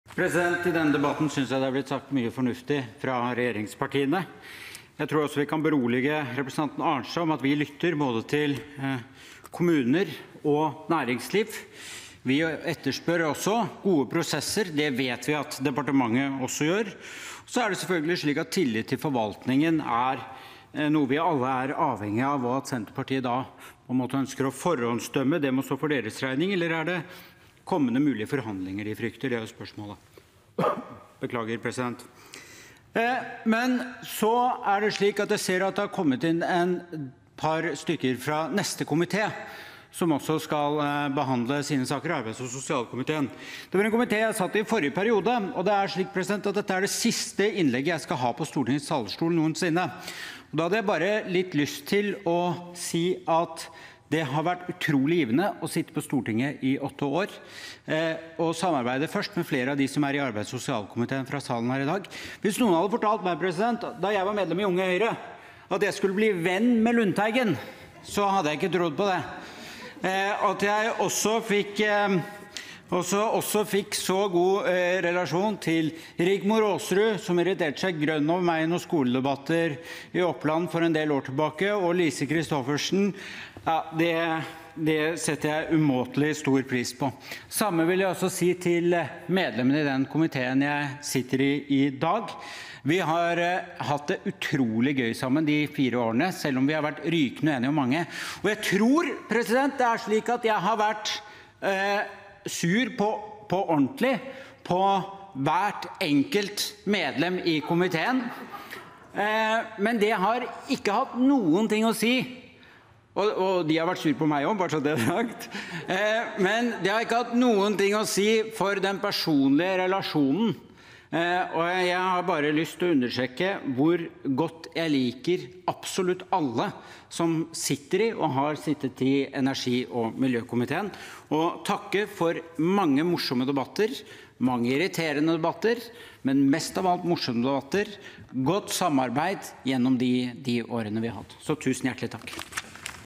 07.06.2021 Sak 22 2 Stefan Heggelund
Om Heilskapleg nasjonal plan for bevaring av viktige område for marin natur – jf. Meld. St. 29 (2020–2021), og om å avslutte statens arbeid med marint vern av fjordarmen Børgin i Trondheimsfjorden (Innst. 557 S (2020–2021), jf. Meld. St. 29 (2020–2021) og Dokument 8:182 S (2020–2021))